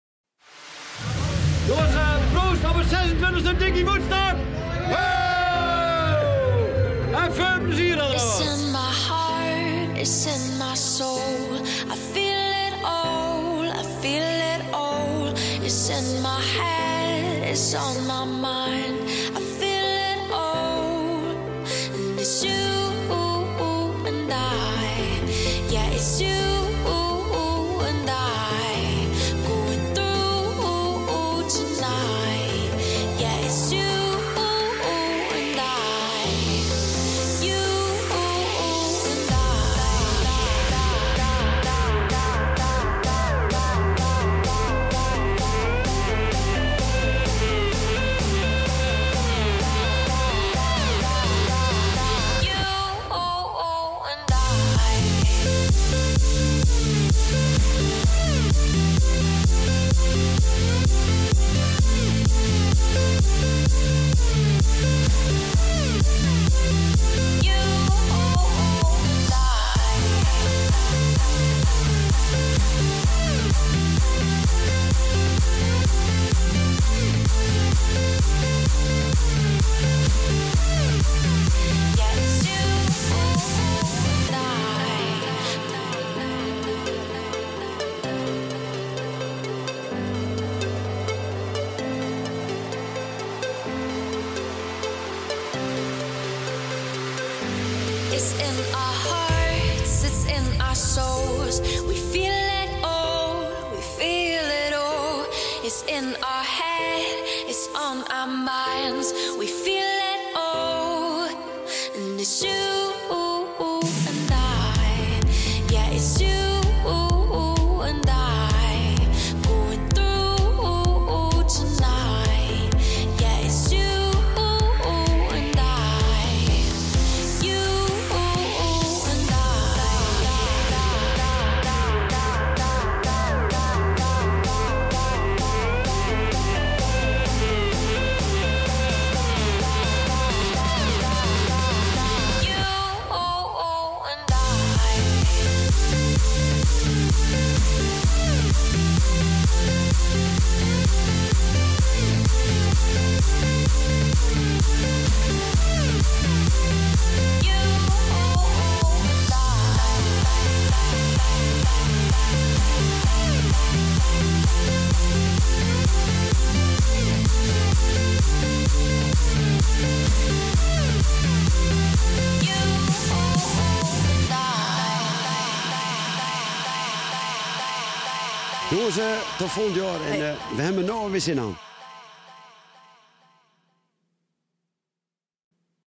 Dicky Woodstock 2014 - Aftermovie
Beleef nog een keer alle hoogtepunten van het Dicky Woodstock Festival 2014. Hopelijk heb jij ook genoten en zien we je volgend jaar weer!!